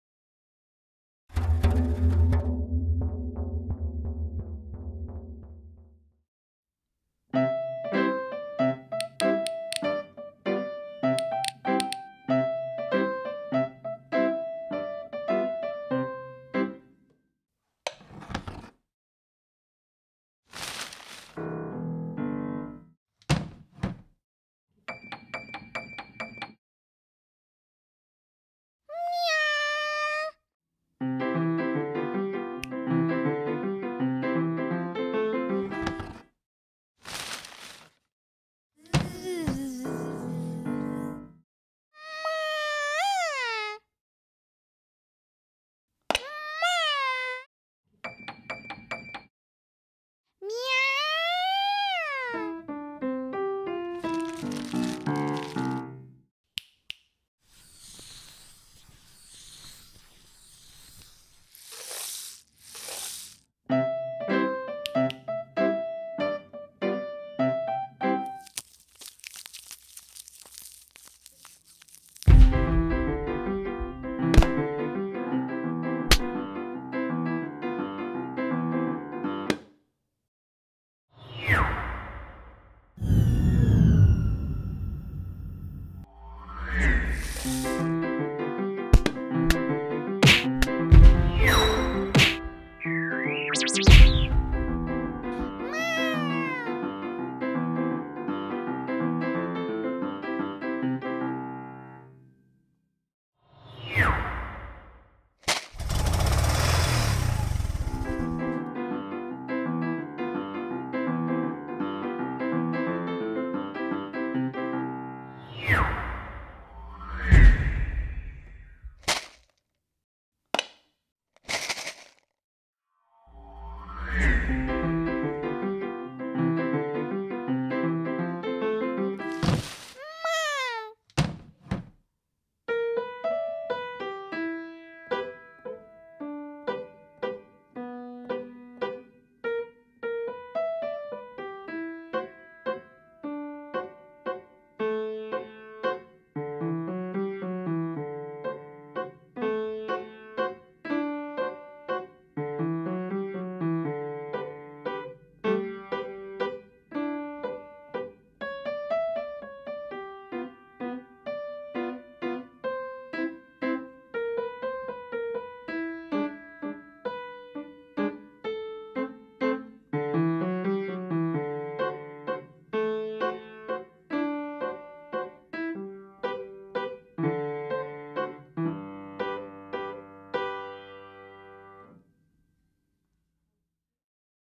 Kat in de Bak.
Een stop-motion film gemaakt door de leerlingen van het zesde leerjaar uit de gemeenteschool van Heestert.  Gedurende 3 dagen hebben deze 18 kinderen vol enthousiasme gewerkt aan deze animatiefilm onder begeleiding van Lynn Vanden Broecke en Lief Vandervoort van Kidscam vzw. .